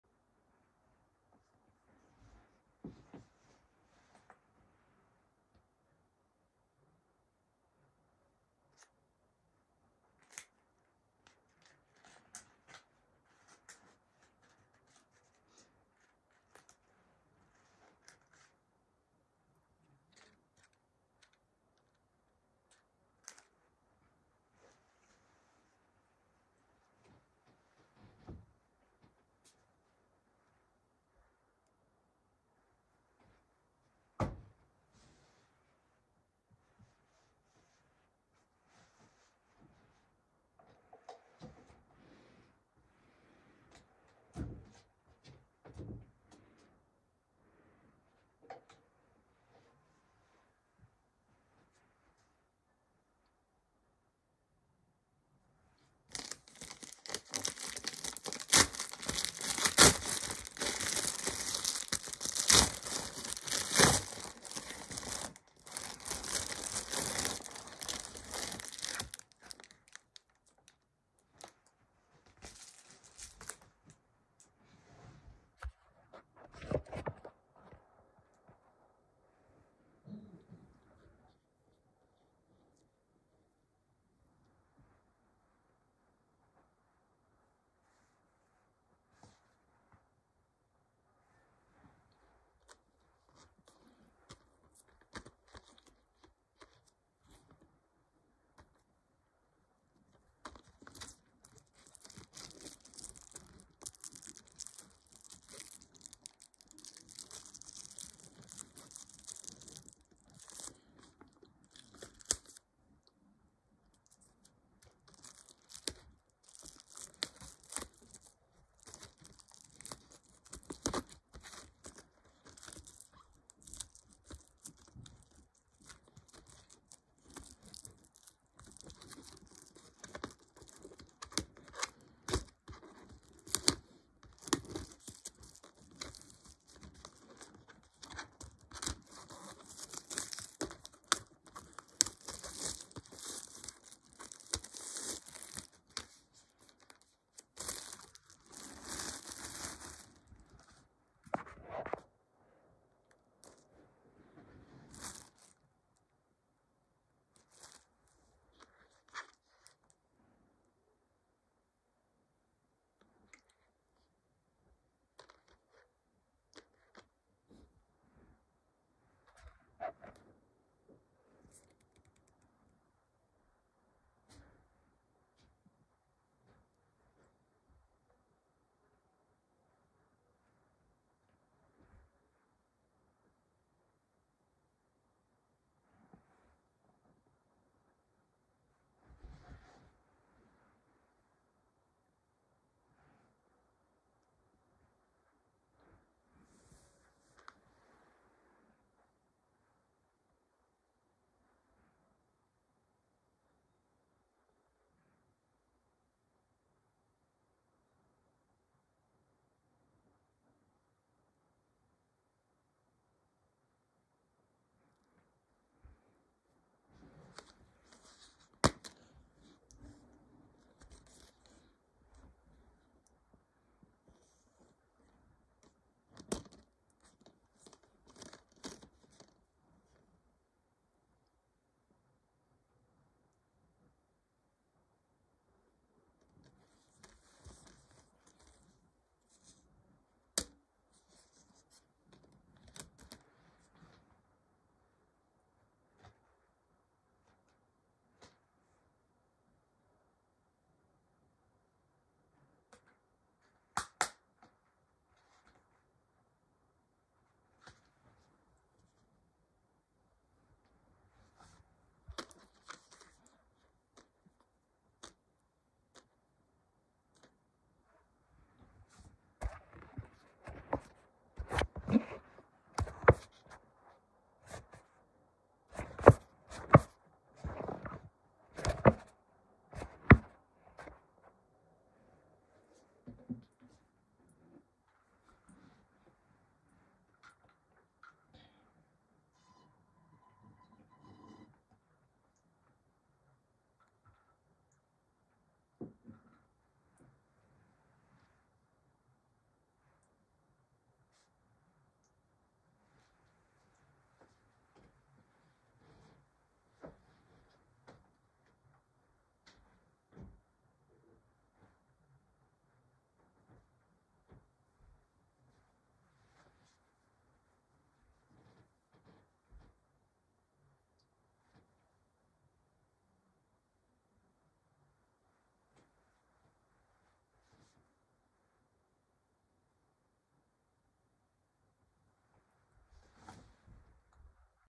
Unboxing Pokémon Detective Pikachu Special Edition DVD
Look what finally arrived this morning from Amazon and not tomorrow!! I was getting ready and leaving for  work my first 11 - 5 shift until I saw It at the front door!